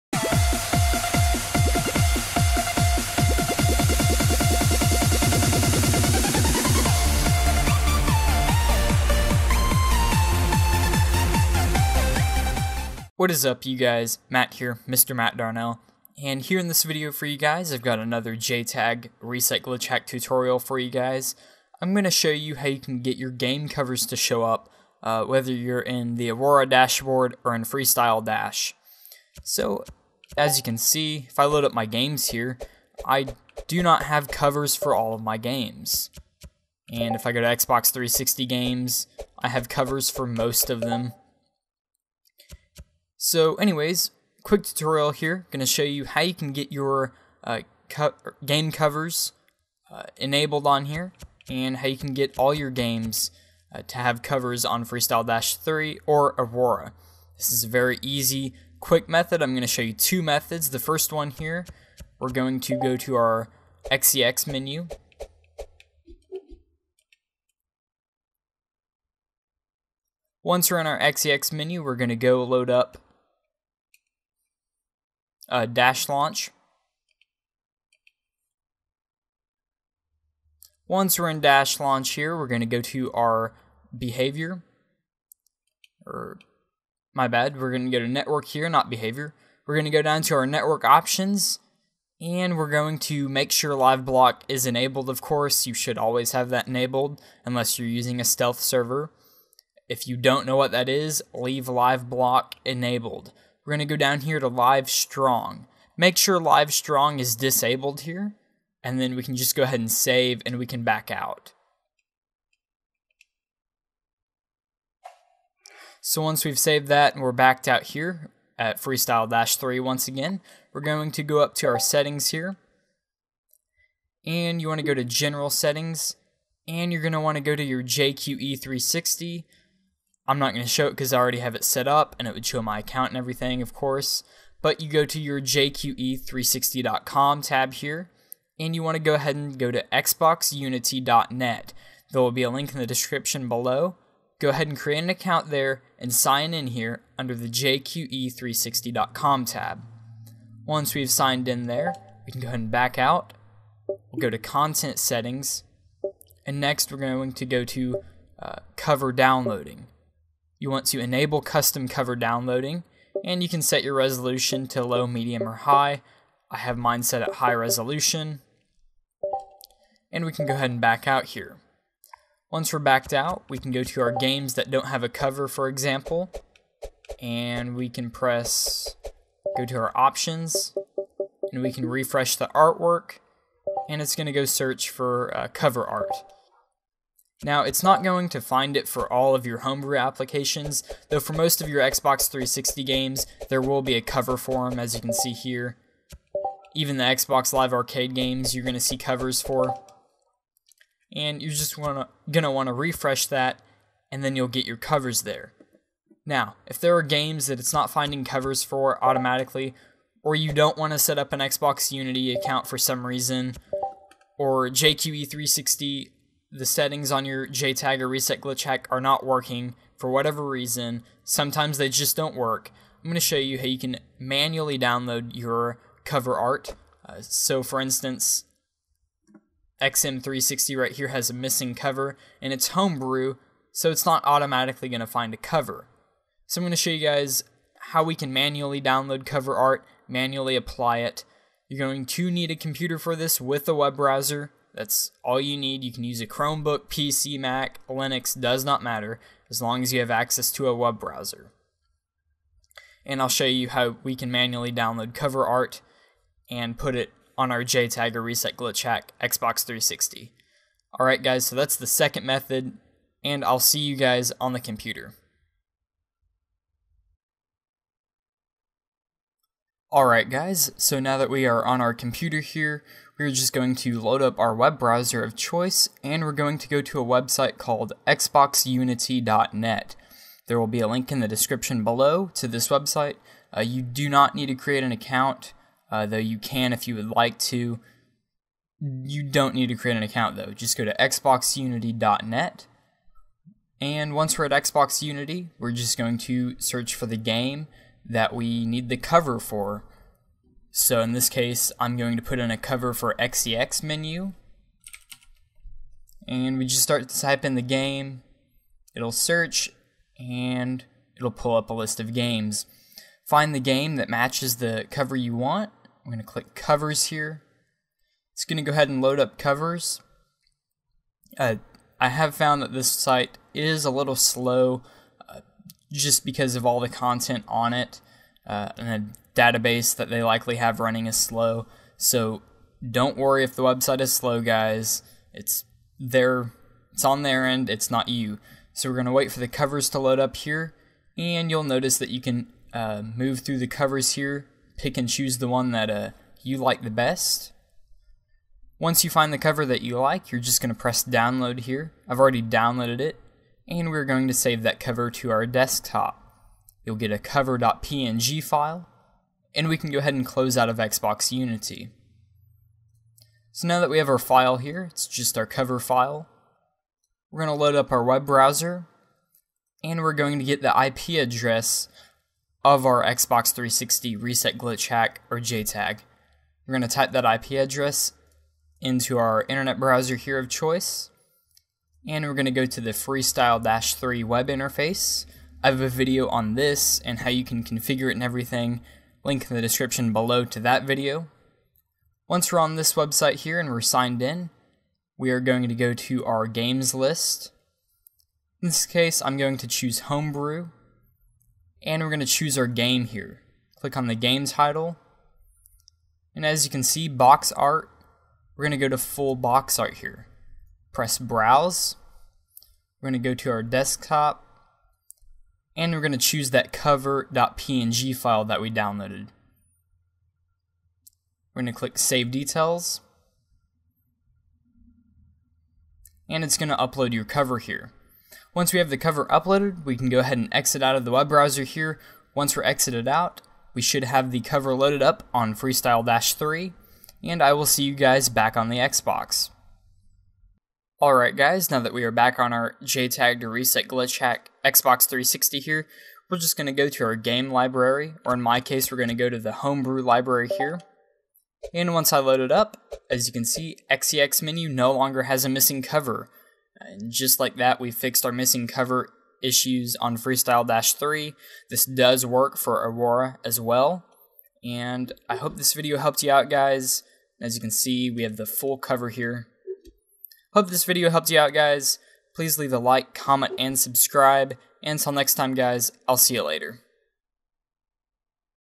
How to Get Cover Art FreeStyle Dash 3 (RGH/JTAG)
↓Click to Expand for Links, Specs, and More Information↓

NOTE: You WILL need a RGH Xbox 360, or a JTAG Xbox 360 as well as XeXMenu with FreeStyle Dash 3 installed. If you do not have one or XeXMenu and FreeStyle Dash 3 installed than this WILL NOT WORK

Hey guys, in this video I will show you how you can download and get cover art for your Xbox 360 games, emulators, and homebrew programs on your JTAG or RGH Xbox 360.  This video will show both ways that you can add cover art, and I hope that at least one of these works out for you. Hope this video helped you out, and you can find links to everything needed below!

Be sure to visit my Twitch!

This is just a new style of videos I am testing out, so I can get more videos out for you guys! As well as show you what I am up to, and some of the stuff I do at my work.